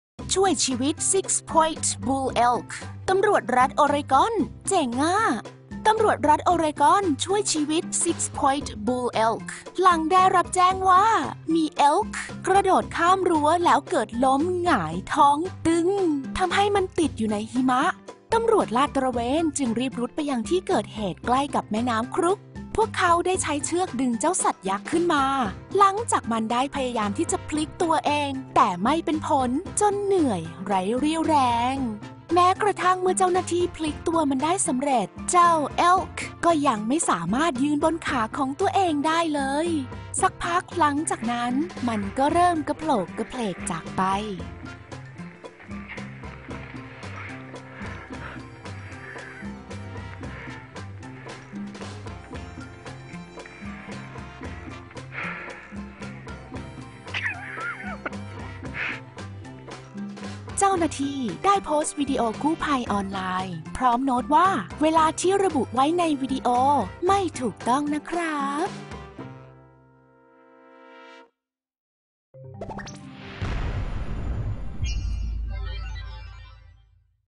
ช่วยชีวิต SIX POINT BULL ELK ตำรวจรัฐโอเรกอนเจ๋ง
PRINEVILLE, OREGON —  ตำรวจรัฐโอเรกอนช่วยชีวิตเหลือ SIX POINT BULL ELK หลังได้รับแจ้งว่ามี elk กระโดดข้ามรั้วแล้วเกิดล้มหงายตึง ทำให้ติดอยู่ในหิมะ ตำรวจลาดตระเวนจึงรีบรุดไปยังที่เกิดเหตุใกล้กับแม่น้ำครุ๊ก

พวกเขาได้ใช้เชือกดึงเจ้าสัตว์ยักษ์ขึ้นมา หลังจากมันได้พยายามที่จะพลิกกลับเองแต่ไม่เป็นผลจนเหนื่อยไร้เรี่ยวแรง  แม้กระทั่งเมื่อเจ้าหน้าที่พลิกตัวมันได้สำเร็จ เจ้า elk ก็ยังไม่สามารถยืนบนขาของตัวเองได้ สักพักหลังจากนั้น มันก็เริ่มกะโผลกกะเผลกจากไป

เจ้าหน้าที่ได้โพสต์วิดีโอกู้ภัย พร้อมโน้ตว่าเวลาในวิดิโอไม่ถูกต้องนะจ๊ะ